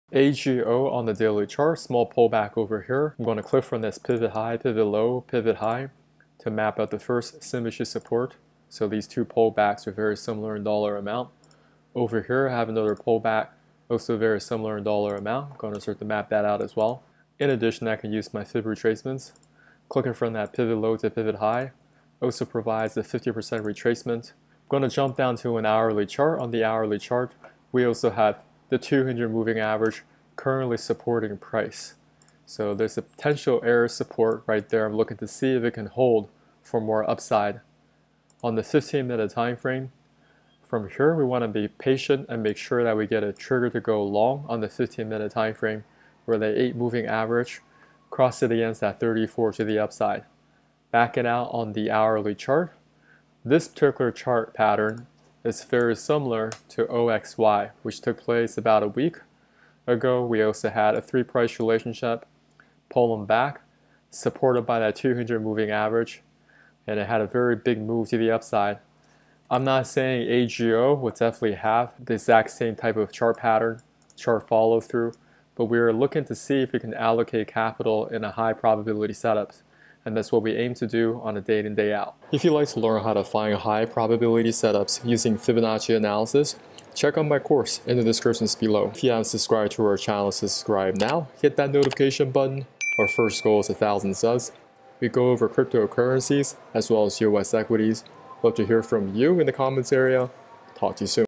Assured Guaranty ($AGO) - Price Pullback to $57.00 Chart Pattern Similar to $OXY. Follow Through? 🚀🚀
💡 BitCoin: 3JQnmjFCMLAkw1yQEA4oVSXk7k4LsiEiWK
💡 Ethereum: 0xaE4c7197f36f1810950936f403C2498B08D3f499

- 𝗖𝗼𝗺𝗽𝗼𝘀𝘂𝗿𝗲
- 𝗖𝗹𝗮𝗿𝗶𝘁𝘆
- 𝗖𝗼𝗺𝗺𝗶𝘁𝗺𝗲𝗻𝘁